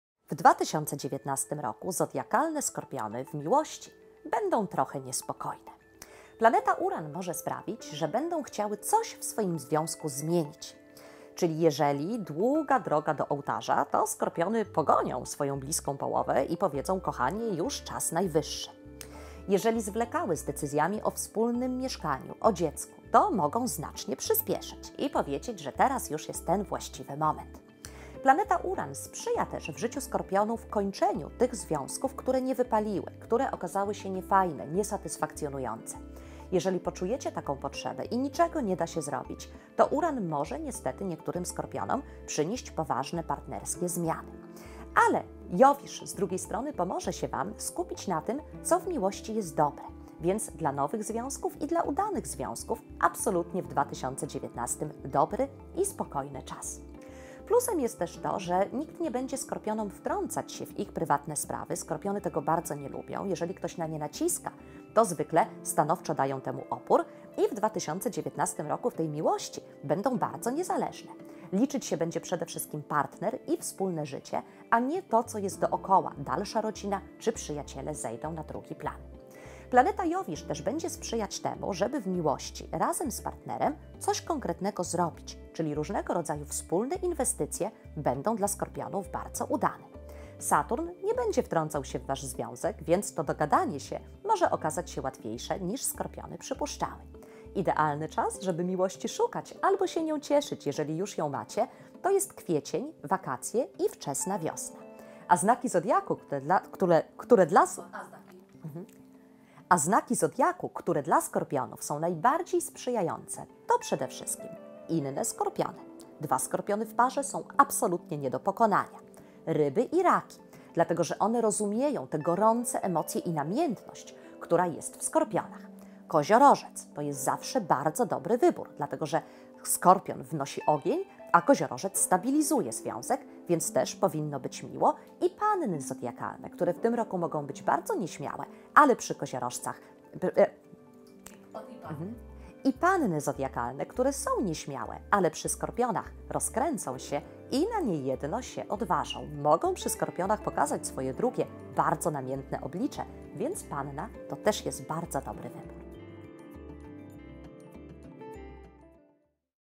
SKORPION - miłosny horoskop roczny
Horoskop Partnerski - Skorpion (kobieta) na 2019
Pani Skorpion jest kobietą pewną siebie i silną. Nie zwraca uwagi na nieśmiałych i skrytych panów. Lubi jednorazowe przygody, które są dla niej czymś naturalnym. To ona decyduje, kto będzie mógł ją poderwać. W sypialni kobieta-Skorpion jest namiętna i nieokiełznana, wobec kochanka ma spore wymagania.

Horoskop Partnerski - Skorpion (mężczyzna) na 2019
Skorpion flirtuje i romansuje, często z kilkoma kobietami jednocześnie, w czym często się gubi. Nie pragnie stabilizacji, jest bigamistą. Jest sprytny, inteligentny i czarujący. Mężczyzna-Skorpion ma duże doświadczenie seksualne. Lubi kochanki odważne, otwarte na rozmaite doświadczenia.

OBSERWUJ NAS NA SNAPIE: follow.gazeta